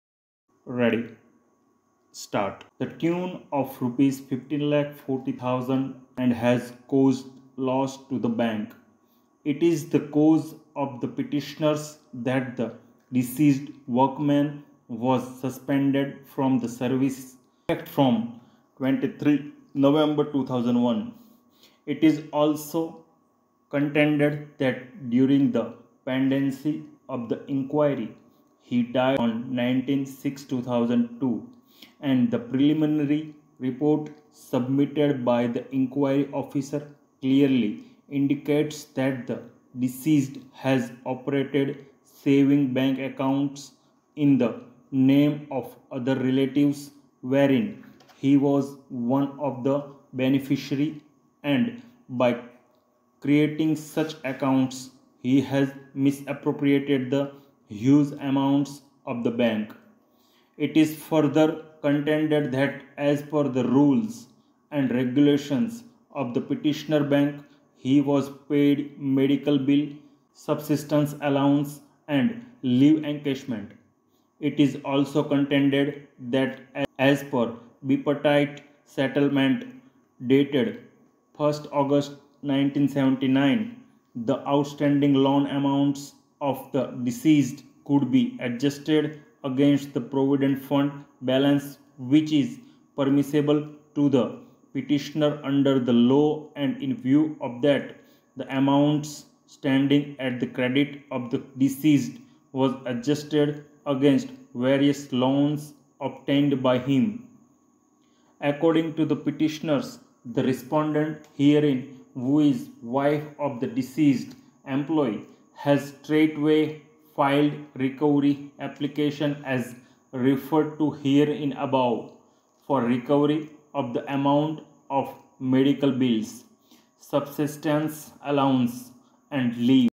120 wpm/ M / gujarat High court Steno / Short-Hand Dictation legal matter / dictation
Transcript this Short-hand Dictation matter and send it to our telegram channel with your name and transcription  time.
Send your transaction and out-line both in pdf format. Our team will help you out in achieving accuracy. This is absolutely free.to find pdf join our telegram channel
@S.V. Steno @Shorthand Dictation Legal Matters @Shorthand Dictations GUJARAT HIGH COURT